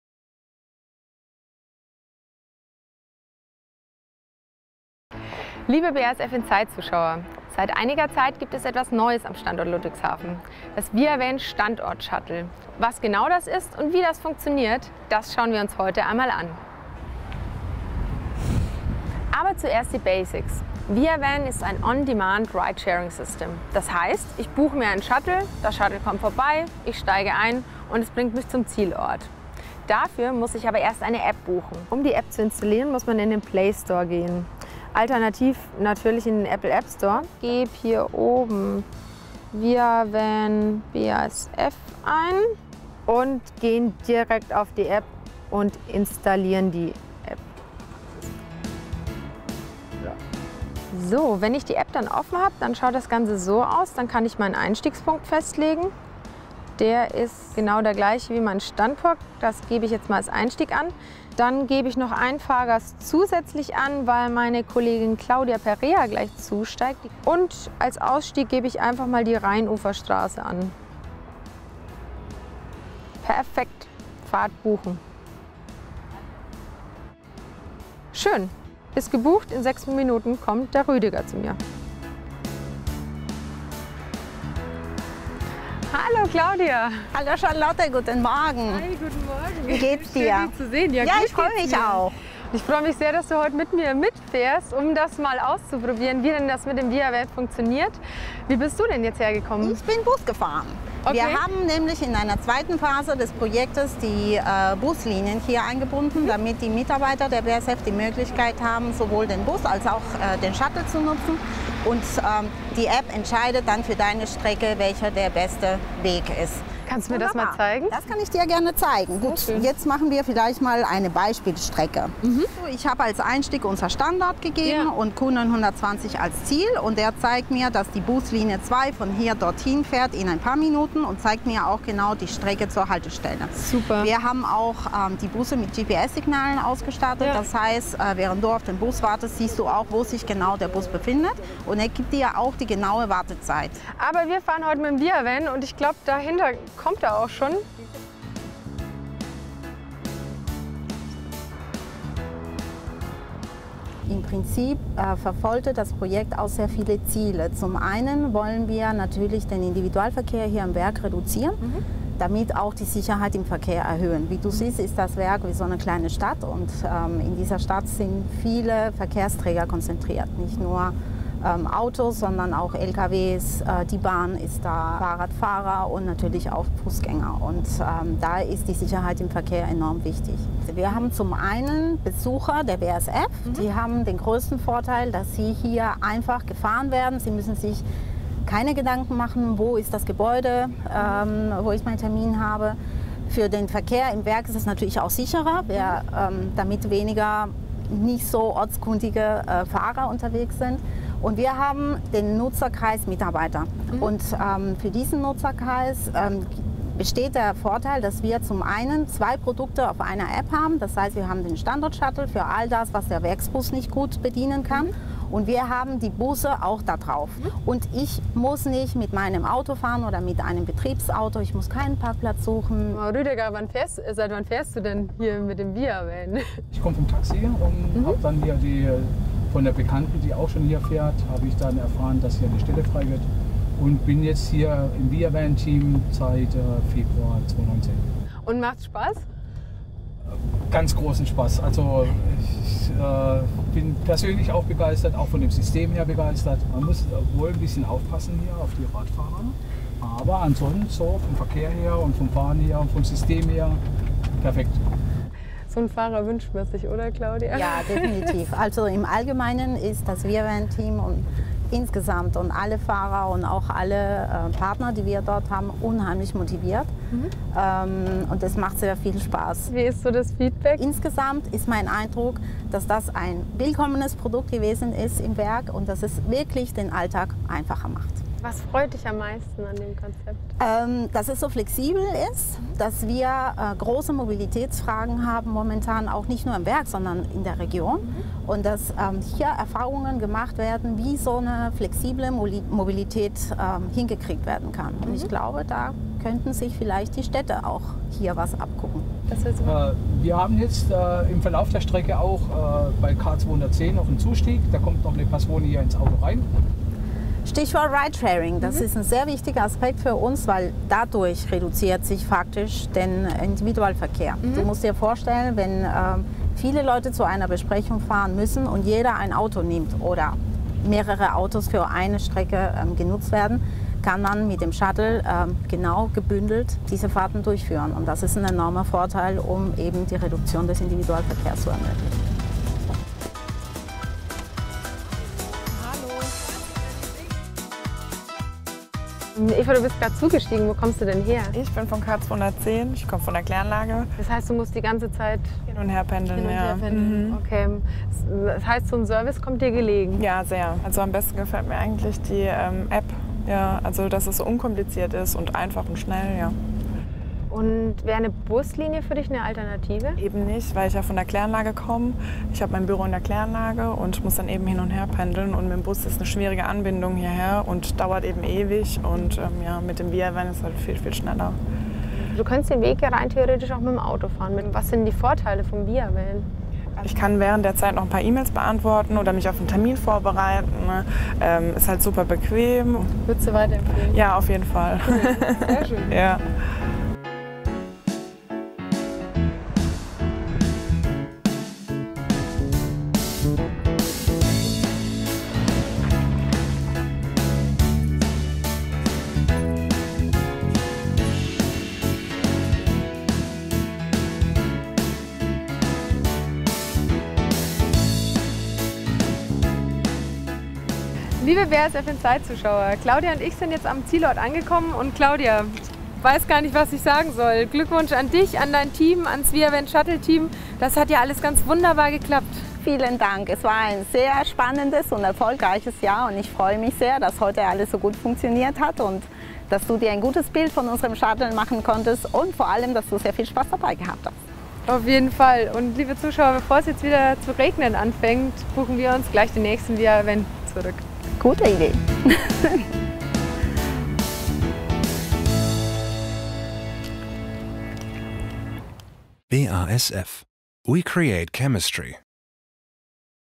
ViaVan Standort Shuttle: Innovative Mobilität
Das Standort Shuttle Ludwigshafen ist ein App-basierter Mobilitätsservice für BASF-Besucher und Mitarbeiter. Einfach App installieren, Fahrt buchen und schnell und sicher zum werksinternen Ziel gelangen.
Das ‚BASF inside‘ Team hat den Service ausprobiert, mit der BASF-Verantwortlichen für das Projekt, dem Shuttle Fahrer und Mitfahrern gesprochen.